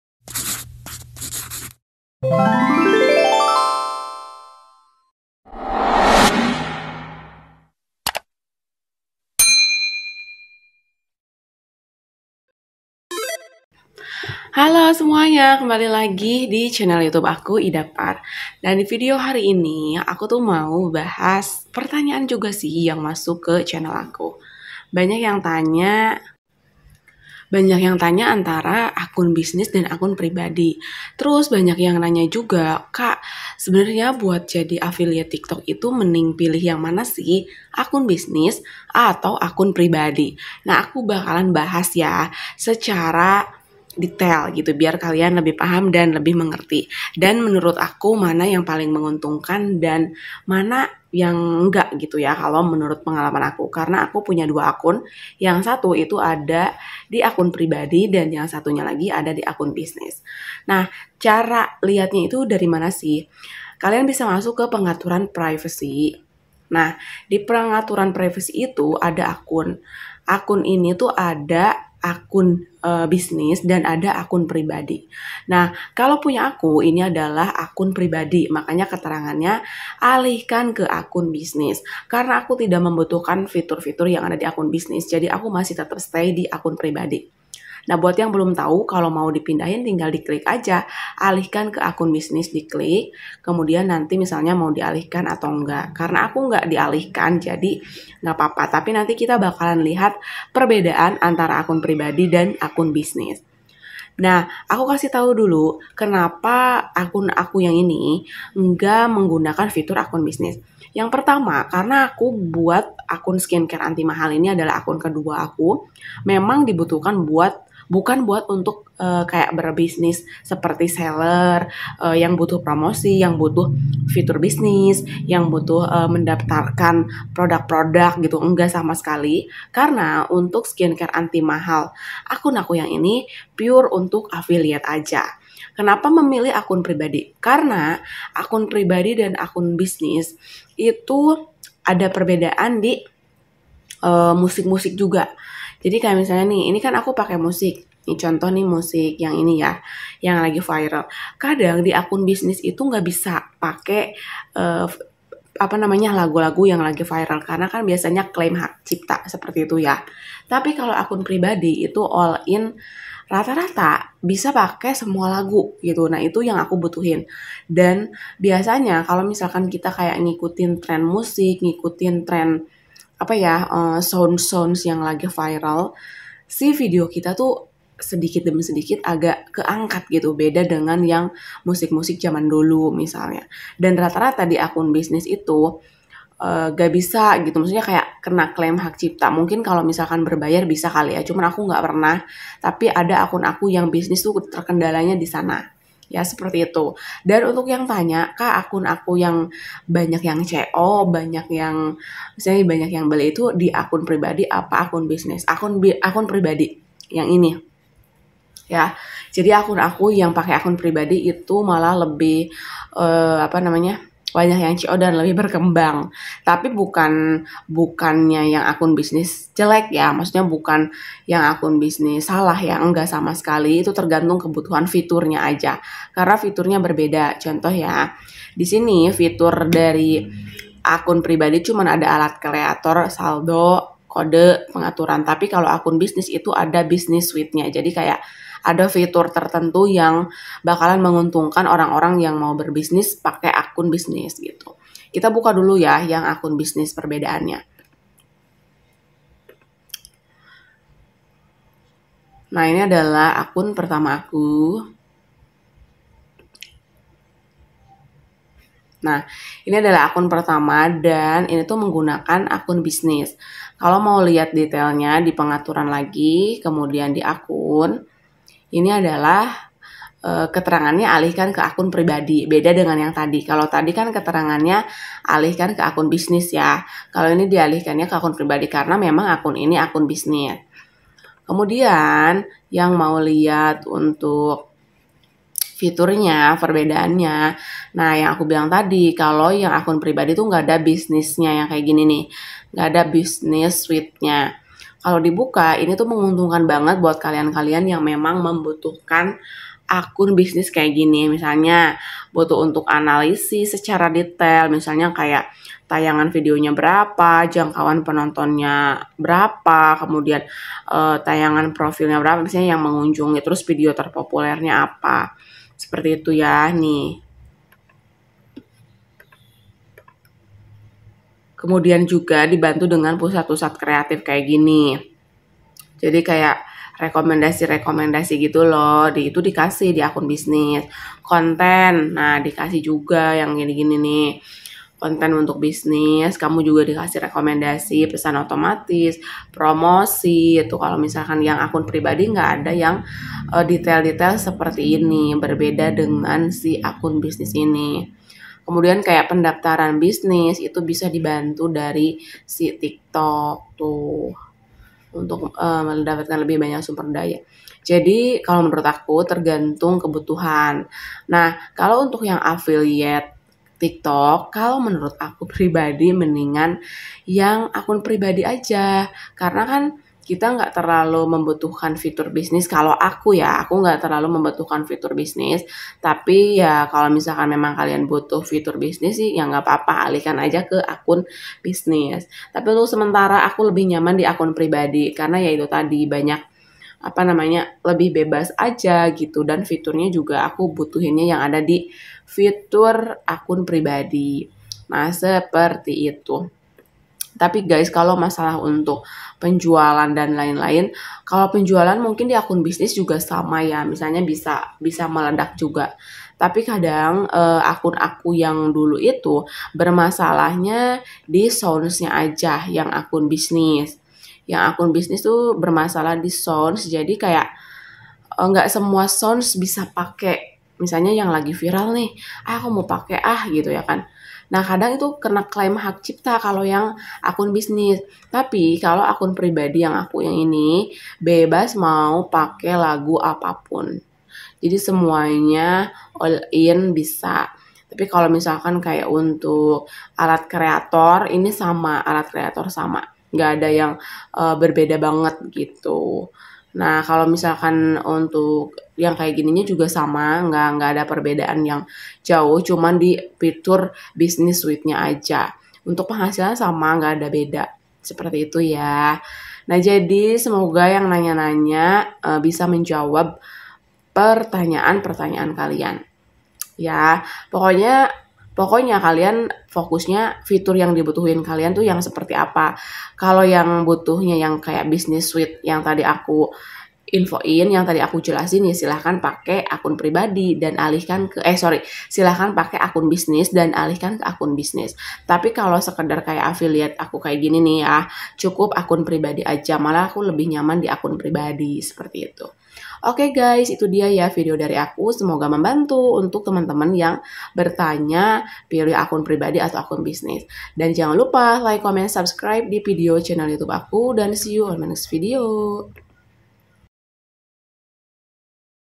TIKTOK AFFILIATE PAKE AKUN PRIBADI / AKUN BISNIS ✅ LEBIH MENGUNTUNGKAN YANG MANA ✅ Akun ku pake ini
Kode Tim:
4XGK2TV